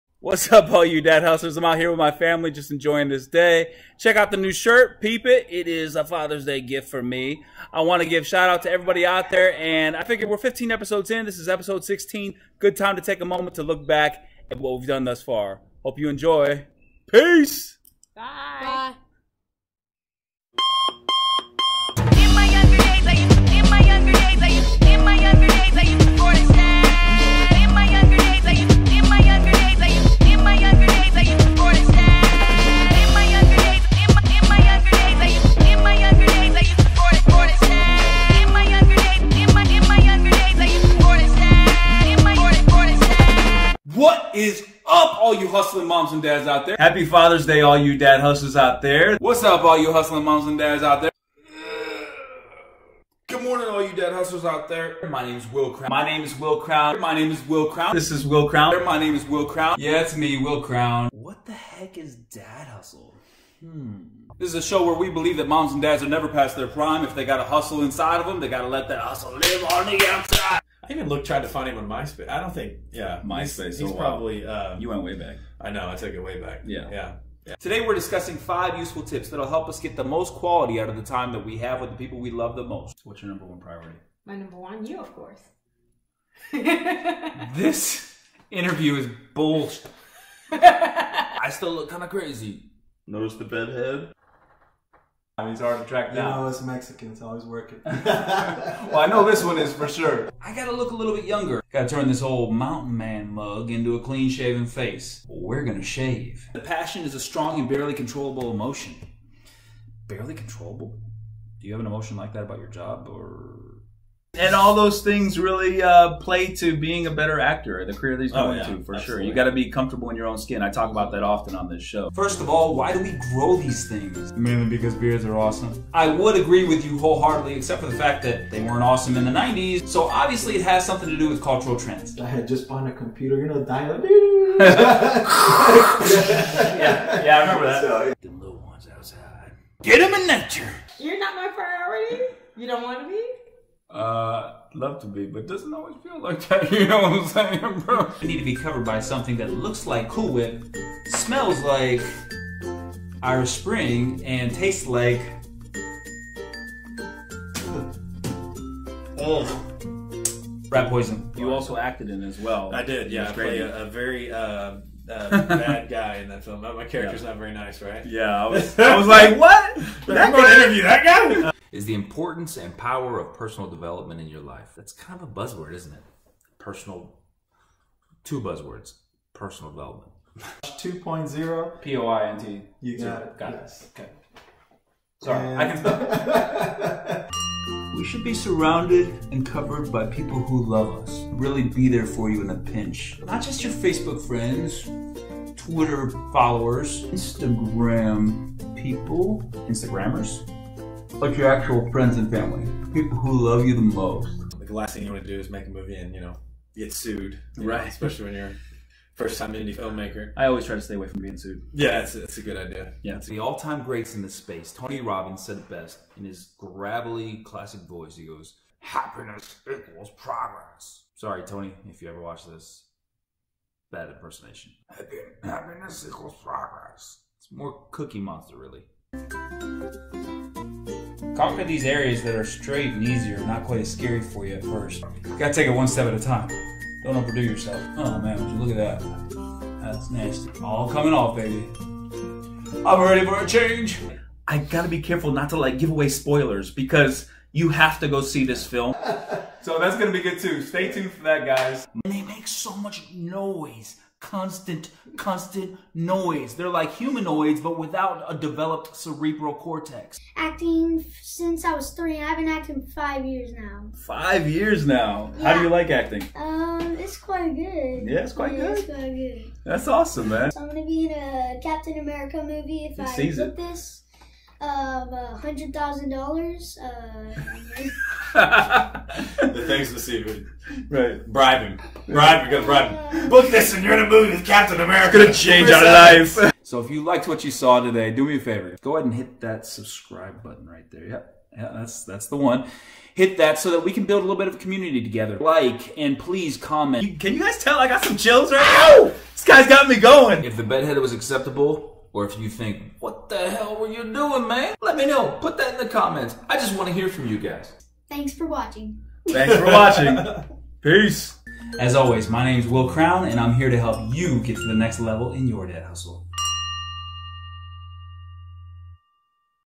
DAD HUSTLE, EP.16 - COMPILATION // Wil Crown
Let's take a look back at the first 15 episodes of DAD HUSTLE!
I LOVE doing LIFE with all of You!!!